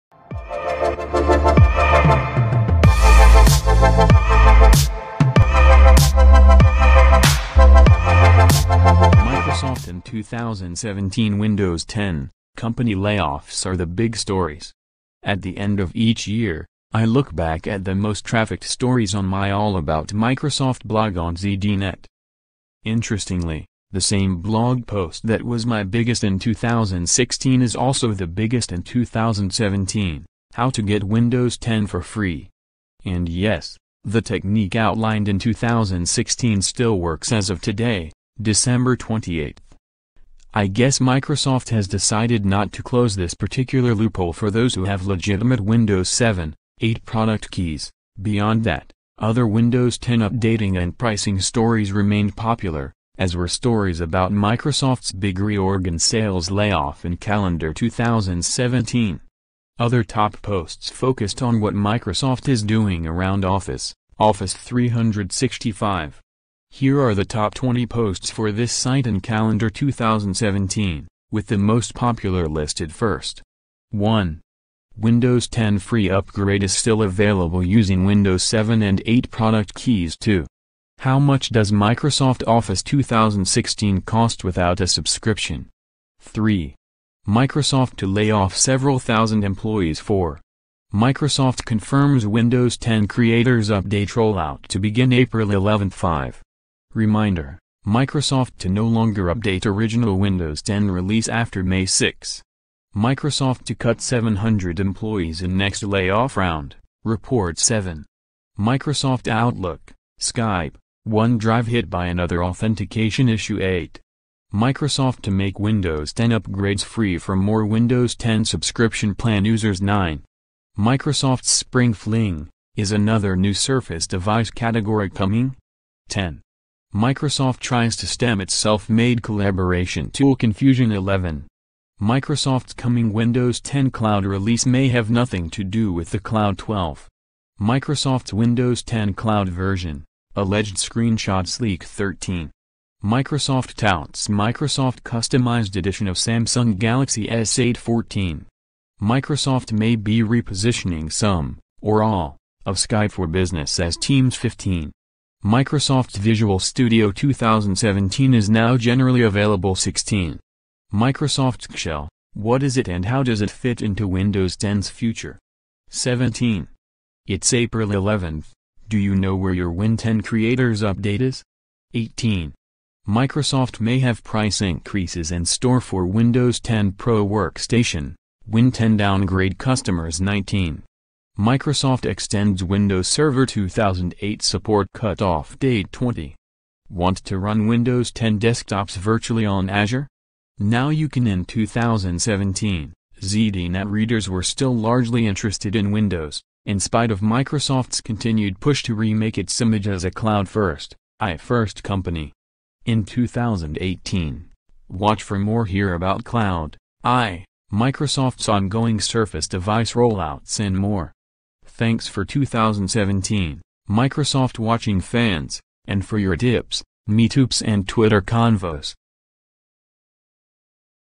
Microsoft in 2017 Windows 10, company layoffs are the big stories
At the end of each year, I look back at the most trafficked stories on my "All About Microsoft" blog on ZDNet.

Interestingly, the same blog post that was my biggest in 2016 is also the biggest in 2017: How to get Windows 10 for free. And yes, the technique outlined in 2016 still works as of today, December 28. (I guess Microsoft has decided not to close this particular loophole for those who have legitimate Windows 7, 8 product keys.)

Beyond that, other Windows 10 updating and pricing stories remained popular, as were stories about Microsoft's big reorg and sales layoff in calendar 2017. Other top posts focused on what Microsoft is doing around Office/Office 365.

Here are the top 20 posts for this site in calendar 2017, with the most popular listed first.